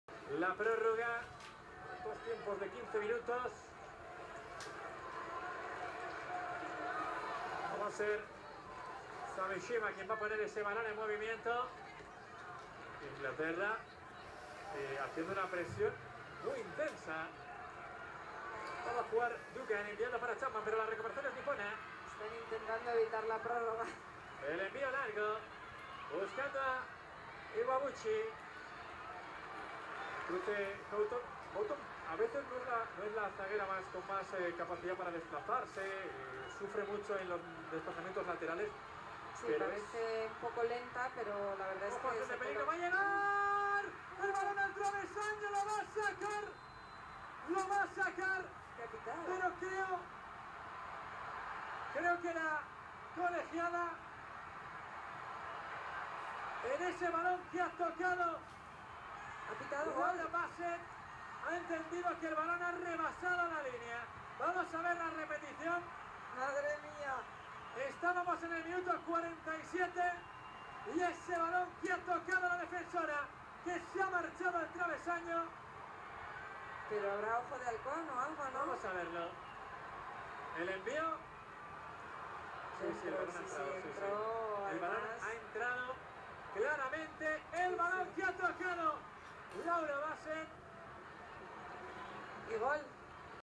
なでしこジャパンvsイングラント ワールドカップ準決勝 オウンゴール FIFA女子ワールドカップ2015
・[永久保存版] なでしこジャパン ドイツＷ杯ハイライト "Queen Yamato Nadeshiko"